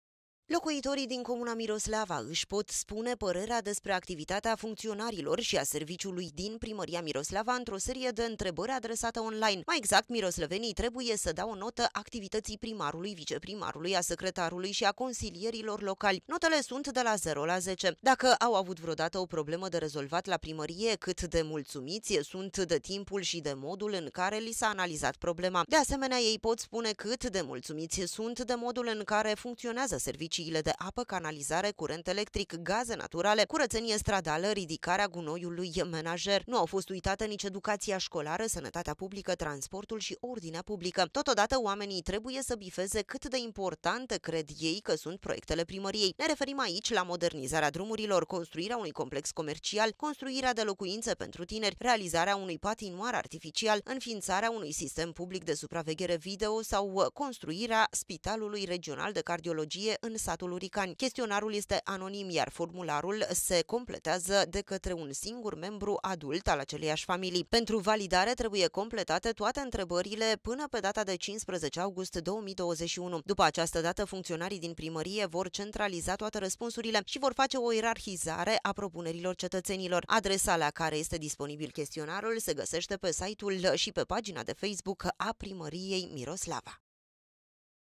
Locuitorii din Miroslava, invitati sa raspunda unui chestionar legat de activitatea Primariei
Administratia locala din comuna Miroslava vrea sa stie cum sunt perceputi functionarii de catre locuitori! In acest sens, conducerea Primariei a lansat un chestionar online. Oamenii pot completa chetionarul pana pe 15 august.